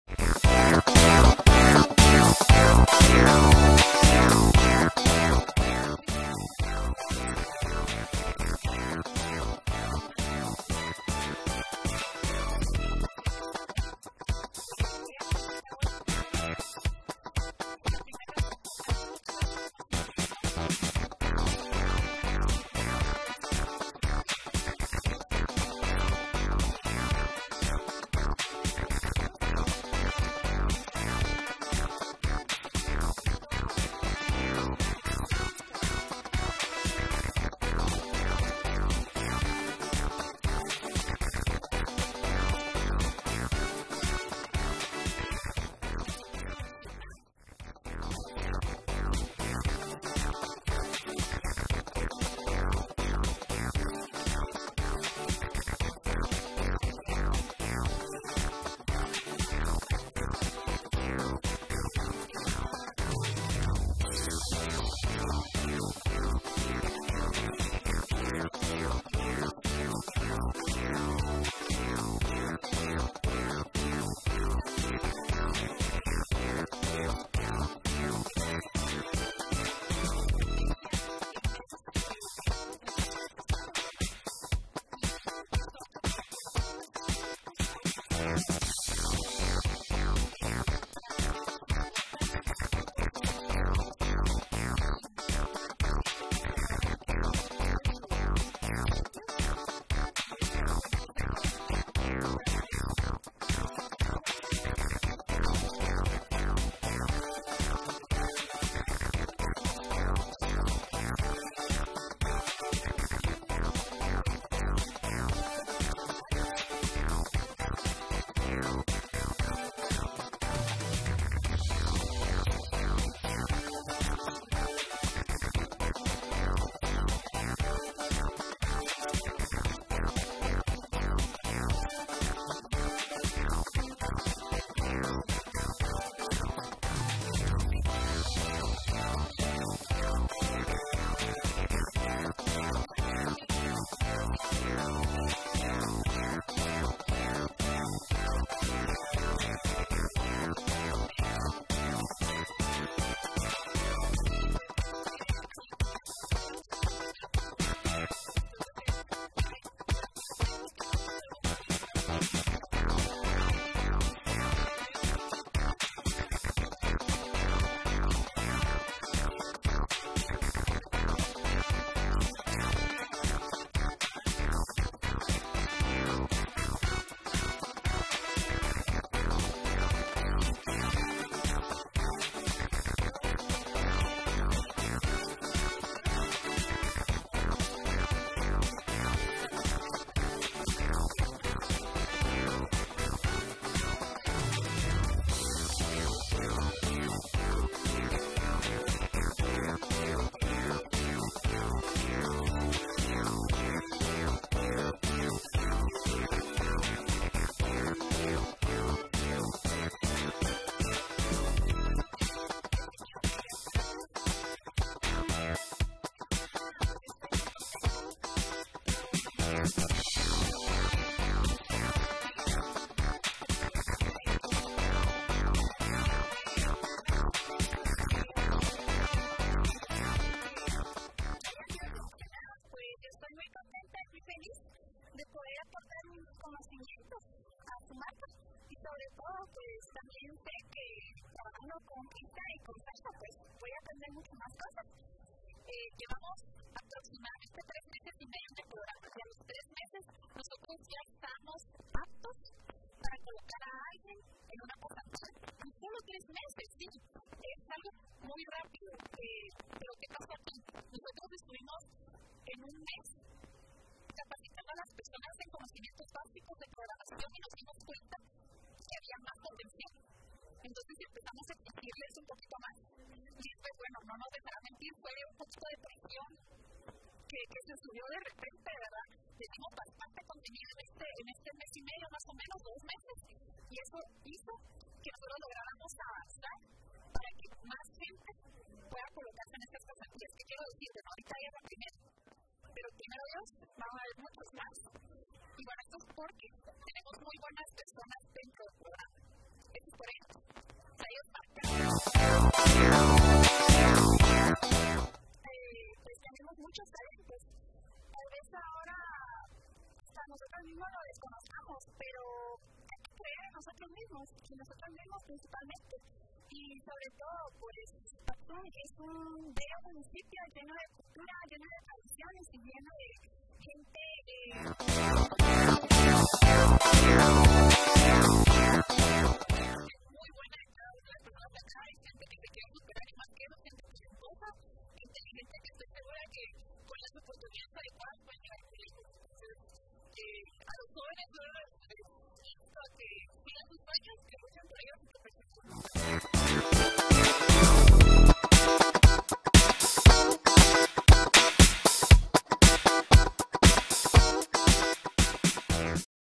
Diseñadora de modas Cristha Fuentes en Patzún
Patzún, con motivo de la incorporación de Lisbeth Mucía, en una pasantía con la diseñadora de moda Cristha Fuentes, como resultado del proceso de formación que un grupo de jóvenes de esta localidad impulsado por el Proyecto denominado "Programa Valentina"